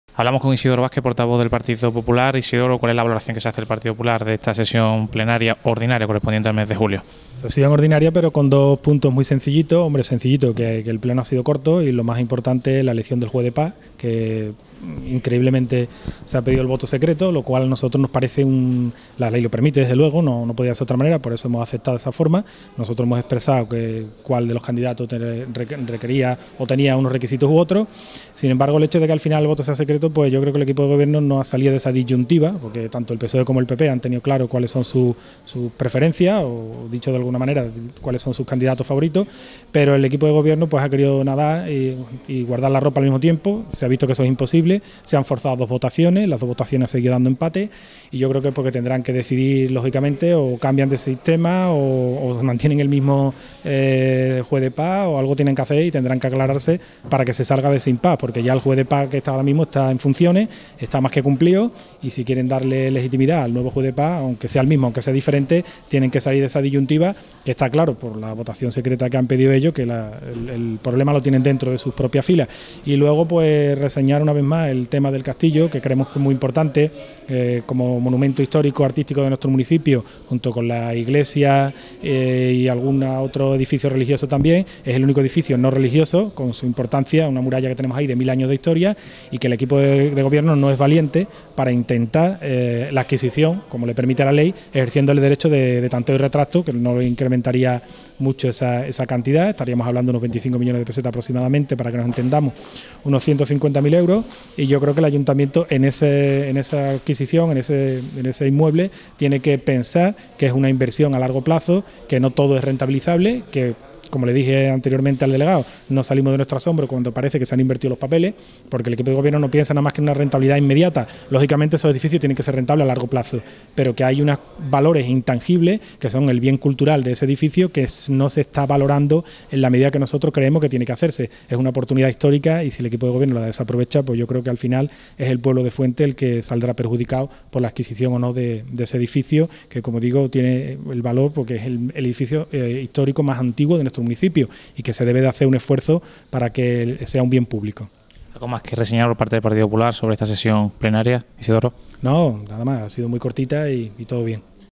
Declaraciones de I. Vázquez (Portavoz PP) Pleno Ordinario Julio 2014
Declaraciones del portavoz del Partido Popular, I. Vázquez, a la finalización del Pleno Ordinario Julio 2014 (Fuentes de Andalucía)